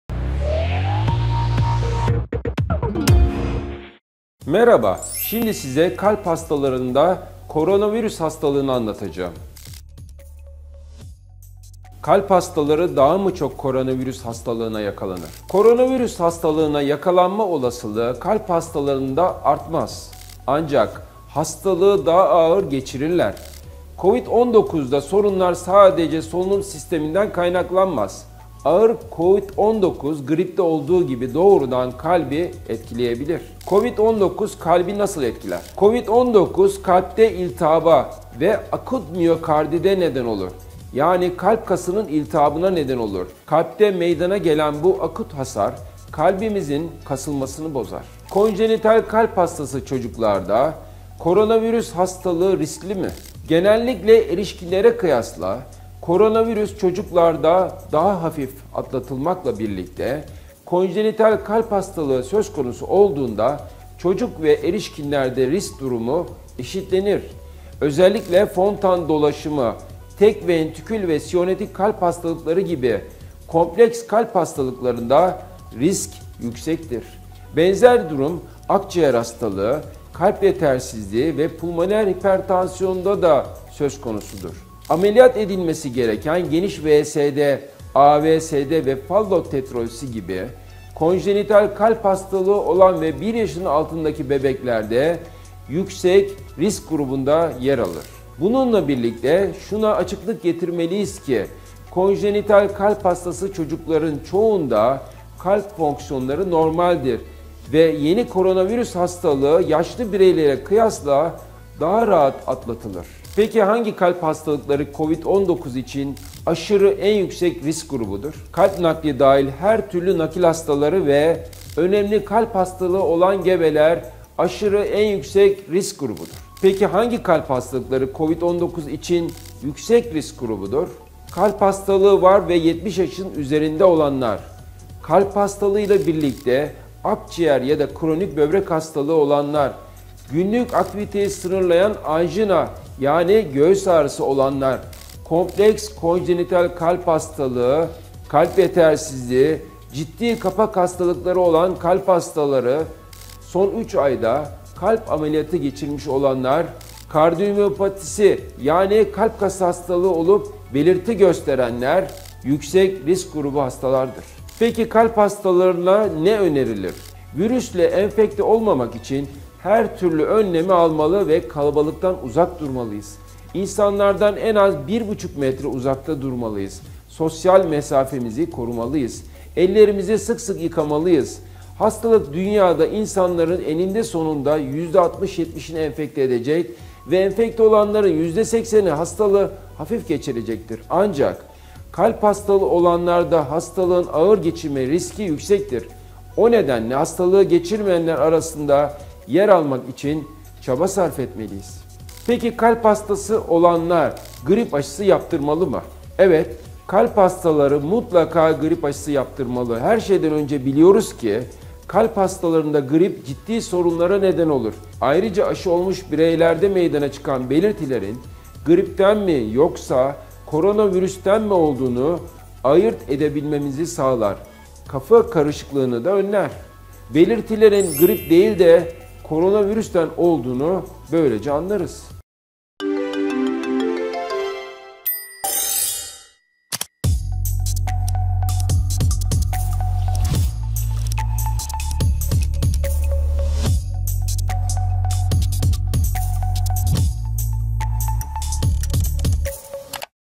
Kalp Hastalarında Koronavirüs | COVID-19
Merhaba size kalp hastalarında koronavirüs hastalığını, COVID-19’u YouTube Kanalımız Hasta Okulu Tv’de anlatıyorum. Bizimle kalın, güncel kalın ❤ Prof. Dr. Taner Yavuz | İstanbul Çocuk Kardiyoloji

Kalp Hastaları Daha mı Çok Koronavirüs Hastalığına Yakalanır?
Koronavirüs Hastalığına yakalanma olasılığı kalp hastalarında artmaz. Ancak hastalığı daha ağır geçirirler. COVID-19 da sorunlar sadece solunum sisteminden kaynaklanmaz. Ağır COVID-19 gripte olduğu gibi doğrudan kalbi etkileyebilir.

COVID-19 Kalbi Nasıl Etkiler?
COVID-19 kalpte iltahaba ve akut miyokardite neden olur. Kalpte akut hasar kalbimizn kasılmasını bozar.

Konjenital Kalp Hastası Çocuklarda Koronavirüs Hastalığı Riskli mi?
Genellikle erişkinlere kıyasla koronavirüs çocuklarda daha hafif atlatılmakla birlikte, konjenital kalp hastalığı söz konusu olduğunda çocuk ve erişkinlerde risk durumu eşitlenir. Özellikle Fontan dolaşımı, tek ventrikül ve siyanotik kalp hastalıkları gibi kompleks kalp hastalıklarında risk yüksektir. Benzer durum akciğer hastalığı, kalp yetersizliği ve pulmoner hipertansiyonda da risk artmıştır.
Ameliyat edilmesi gereken geniş VSD, AVSD ve Fallot tetralojisi gibi konjenital kalp hastalığı olan 1 yaşının altındaki bebeklerde yüksek risk grubunda yer alır.
Bununla birlikte, şuna açıklık getirmeliyiz ki, konjenital kalp hastası çocukların çoğunda kalp fonksiyonları normaldir ve yeni koronavirüs hastalığını yaşlı bireylere kıyasla daha rahat atlatırlar.

Hangi kalp hastalıkları COVID-19 için aşırı en yüksek risk grubudur?

• Kalp nakil dahil her türlü nakil hastaları
• Önemli kalp hastalığı olan gebeler.

Hangi kalp hastalıkları COVID-19 için yüksek risk grubudur?

• Kalp hastalığı var ve 70 yaşın üzerinde ise
• Kalp hastalığı ile birlikte akciğer ya da kronik böbrek hastalığı
• Günlük aktiviteyi sınırlayan anjina
• Kompleks konjenital kalp hastalığı
• Kalp yetersizliği
• Ciddi kapak hastalıkları
• Son 3 ayda kalp ameliyatı geçirmek
• Kardiyomiyopati (belirtili)

Kalp Hastaları Ne Yapmalı?
Virüsle enfekte olmamak için her türlü önlemi almalı ve kalabalıktan uzak durmalıyız. İnsanlardan en az 1,5 metre uzakta durmalıyız. Ellerimiz sık sık yıkamalıyız. Hastalık dünyada insanların eninde sonunda % 60-70 ‘ini enfekte edecek ve enfekte olanların %80 ‘I hastalığı hafif geçirecek. Ancak kalp hastalığı olanlarda hastalığın ağır geçirilme riski yüksektir. O nedenle hastalığı geçirmeyenler arasında yer almak için çaba sarf etmeliyiz.

Kalp Hastası olanlar grip aşısı yaptırmalı mı?
Evet kalp hastaları mutlaka grip aşısı yaptırmalı. Herşeyden önce biliyoruz ki kalp hastalarında grip ciddi sorunlara neden olur. Ayrıca aşı olmuş bireylerde meydan çıkan belirtilerin gripten mi yoksa koronavirüsten mi olduğunu ayırt edebilmemizi sağlar, kafa karışıklığını da önler. Belirtilerin grip değilde koronavirüsten olduğunu anlarız.
Bizimle kalın, güncel kalın!
Prof. Dr. Taner Yavuz
İstanbul Çocuk Kardiyoloji